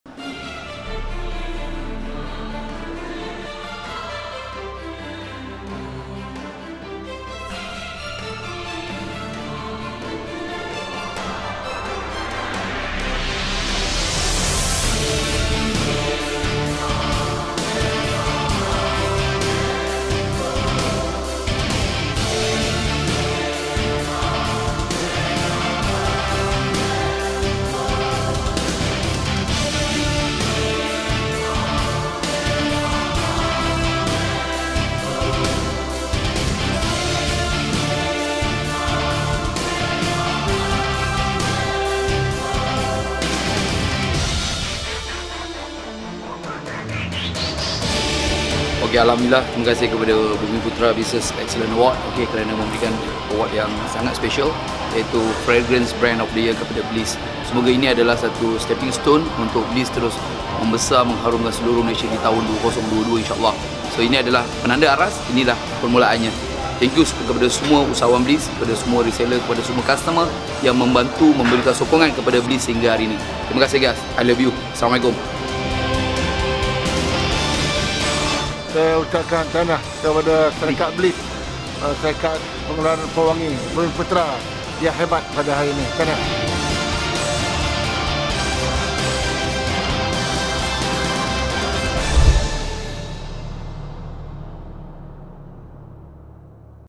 BUMIPUTERA BUSINESS EXCELLENCE AWARD AT SHANGRI-LA HOTEL
A gracious day for us today! We have been awarded as FRAGRANCE BRAND OF THE YEAR 2021 by Bumiputera Business Excellence Award (BBEA). We wish you all thank you for all your support for Bliese to this day.